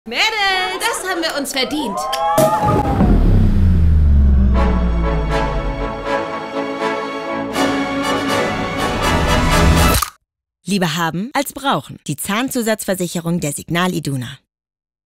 SIGNAL IDUNA „Lieber haben als brauchen“ – Knaller
Lässt Du gern die Korken knallen? Wir auch – zumindest, wenn es nicht so ausgeht wie in unserem Film. Aber falls doch, solltest Du vorher noch eine Zahnzusatzversicherung abschließen.
Denn hier gilt: Lieber haben als brauchen!

Und jetzt haben wir uns den Schampus verdient! Stößchen!